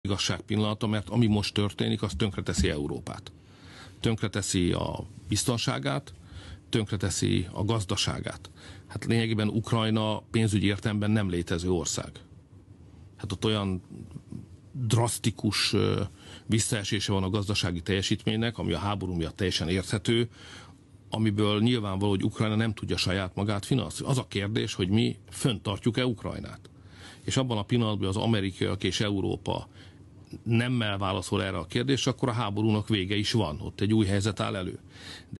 Орбан: Украина в финансовом плане — это несуществующая страна
Как только США и Европа прекратят её содержать, конфликт закончится, заявил Орбан.

Он также добавил, что Европа тратит на содержание киевского режима десятки миллиардов долларов и это не может продолжаться бесконечно